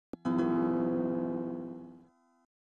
Nobel Tech intro in 4ormulator V1 {Request}
4ormulator V1 for the 01st time!? Awesome! Source by Nobel Tech (Martin Nobel!)
Video source:Microsoft Windows XP Recovery Console startup